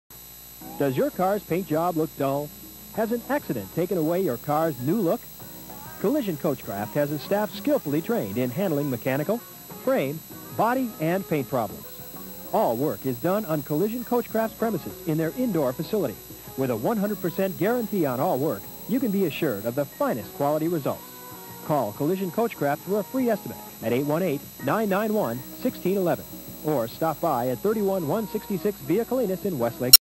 Collision Coach Craft Commercial Ad 1988
Here's A Local Commercial Ad For Collision Coach Craft In Westlake Village.

Voiceover By ?